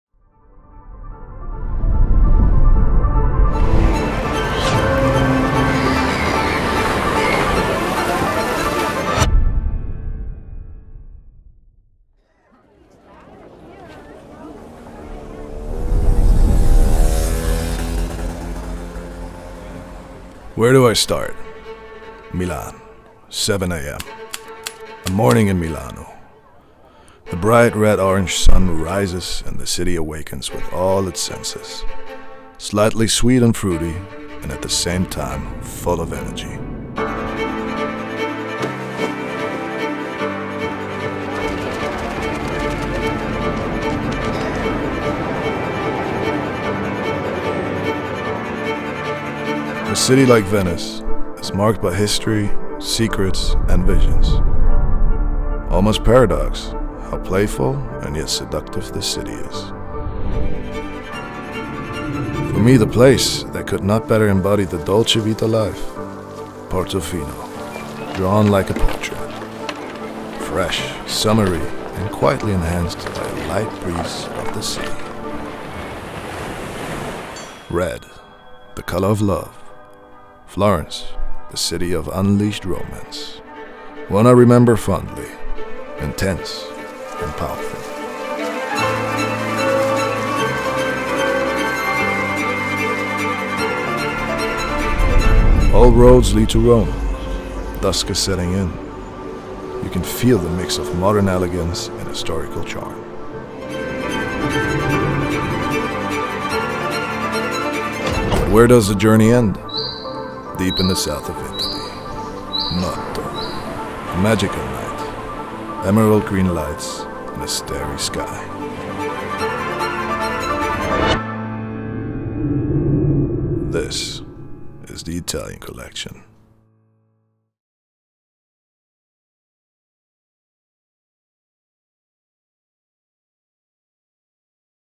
Italian Collection Trailer English
This is the Italian Collection !

Discover the exclusive Italian Collection and join me on a journey to the unforgettable moments and breathtaking backdrops of Italy.
Six new creations let you discover the colourful facets of Italy - from the deep orange of Milan's sunrise to the emerald lights of Noto's night.

Six new compositions. One journey. Your Italian Collection.